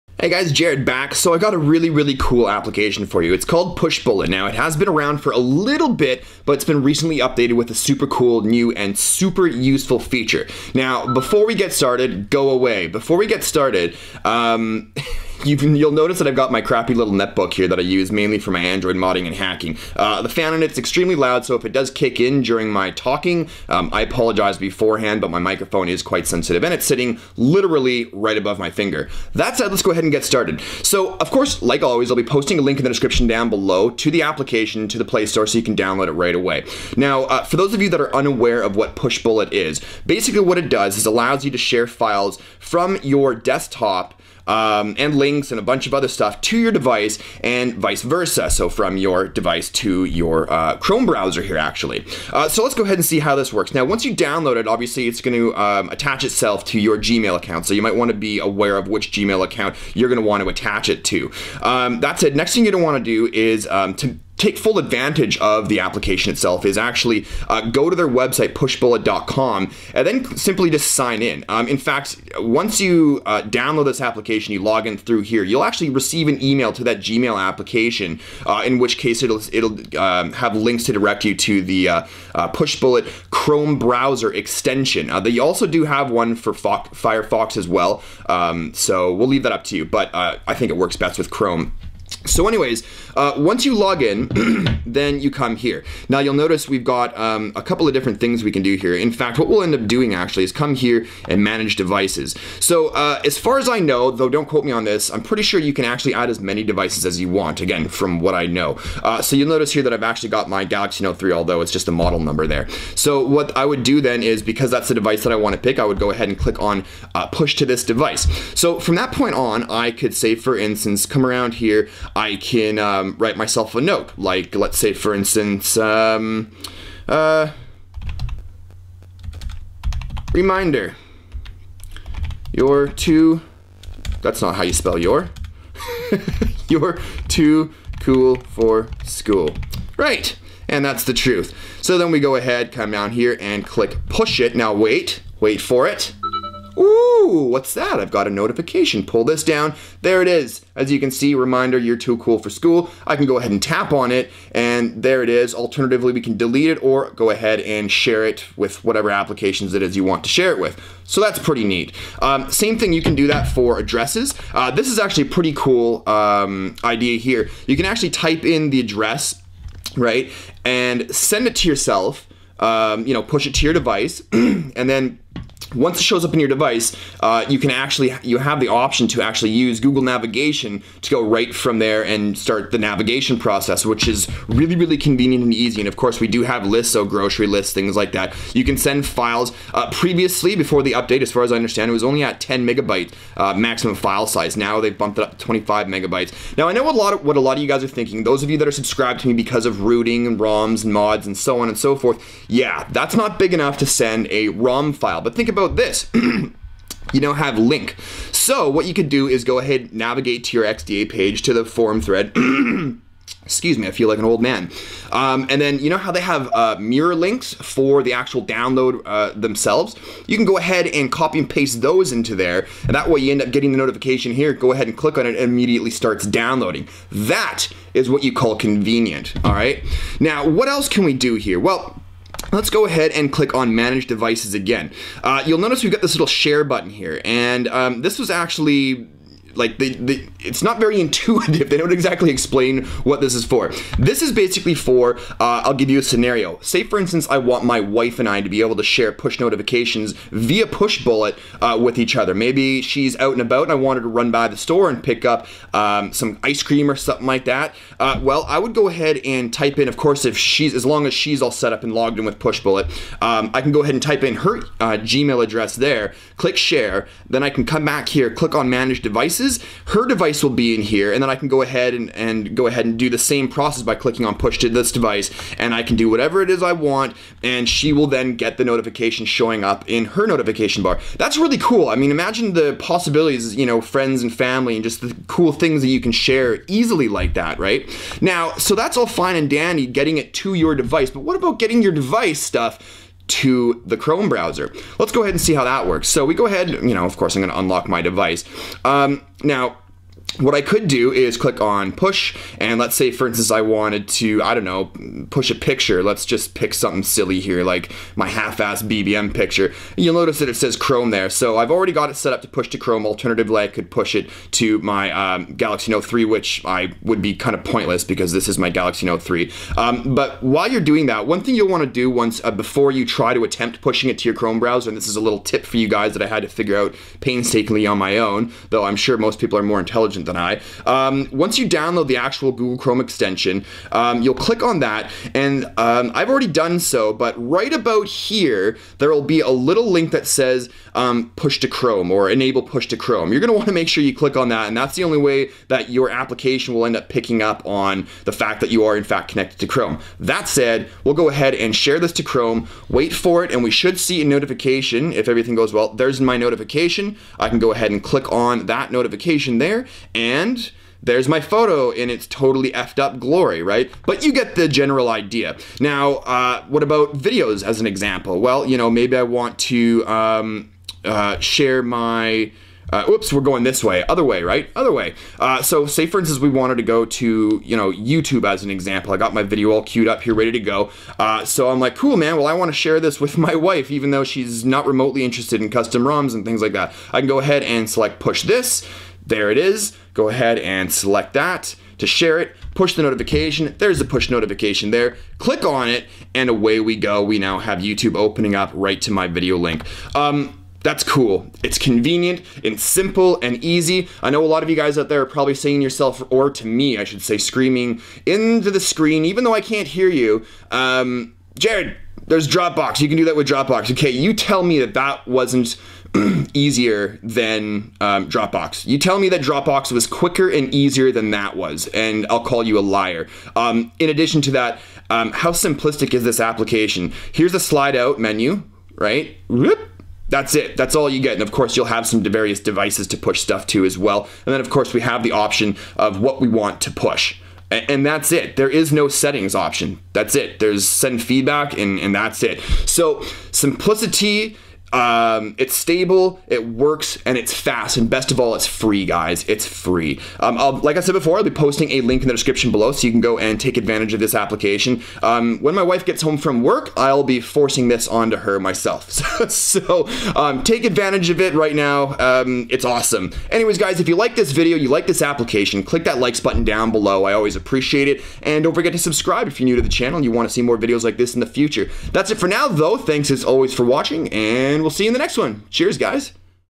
PUSH BULLET Android App Review & How To
PushBullet has recently been updated to not only allow you to send files from your browser to your device but also from your device to your browser, and anyone else as well!